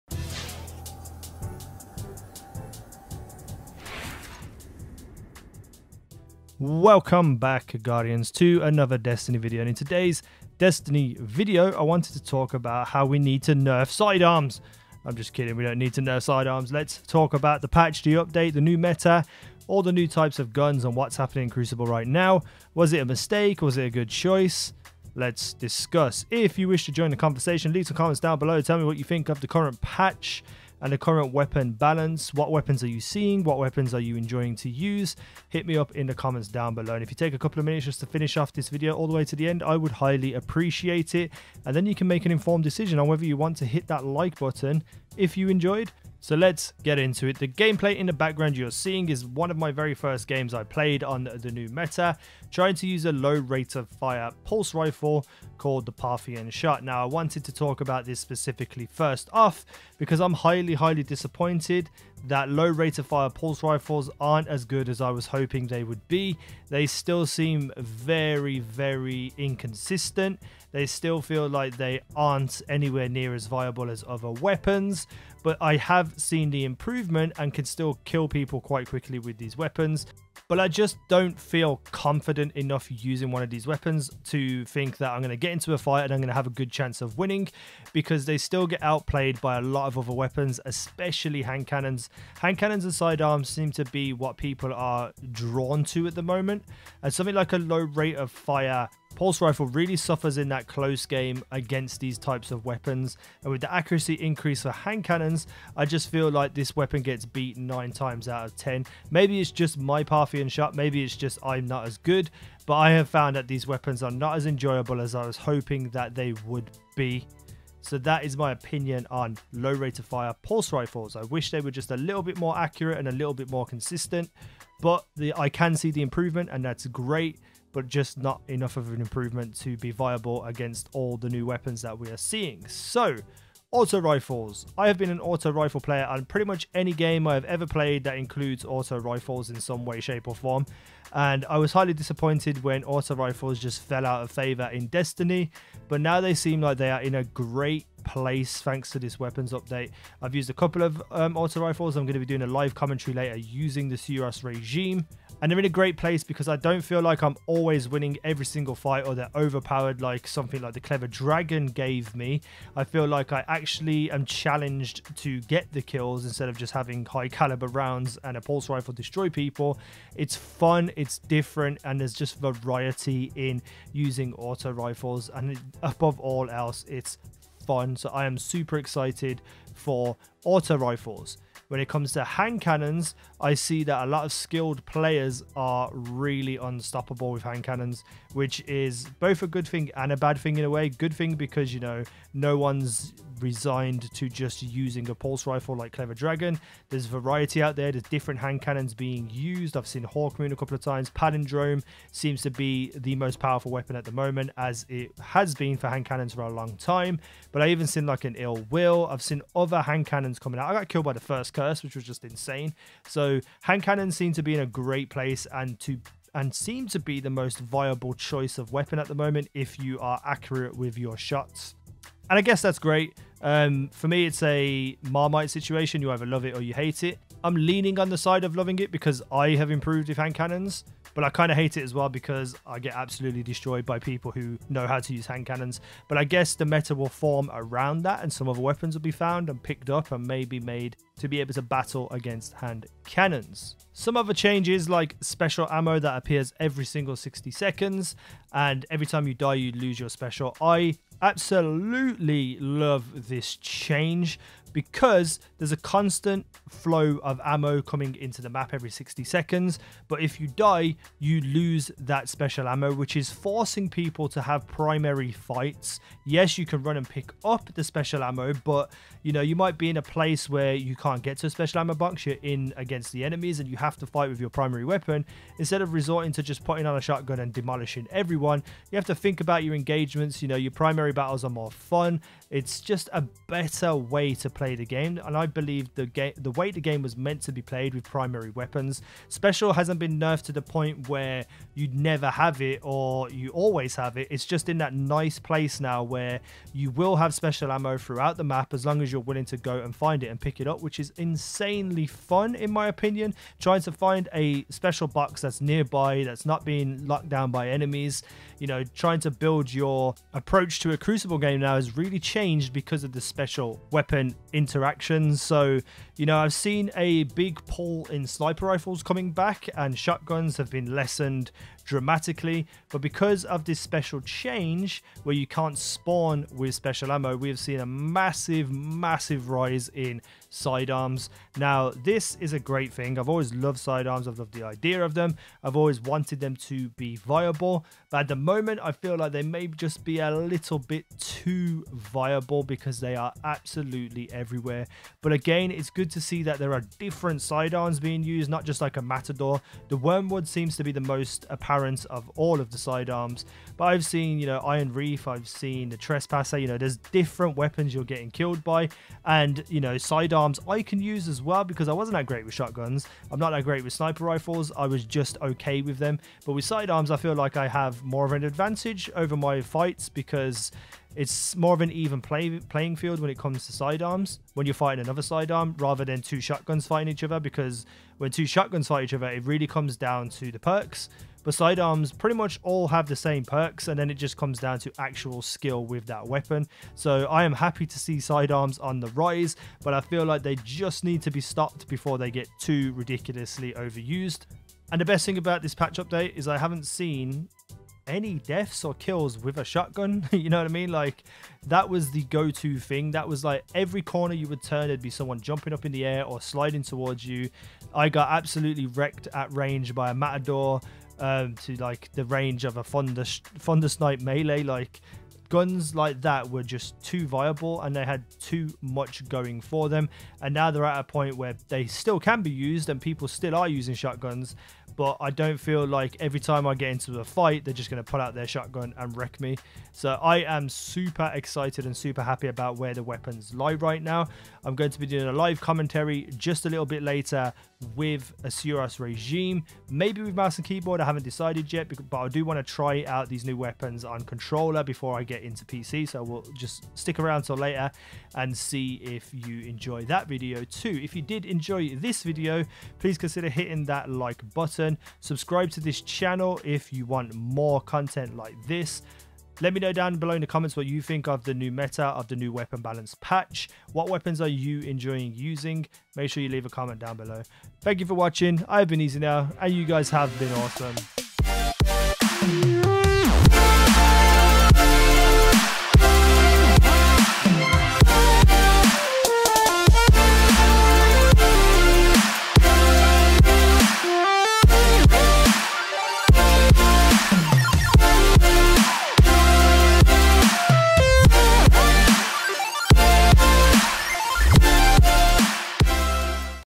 Destiny Weapons Balance Patch 2.5.0.2 - Primary META! Good Or Bad?
I HAVE SPENT OVER $1000 on this game! ● Destiny weapon balance and patch 2.5.0.2 is it good or bad?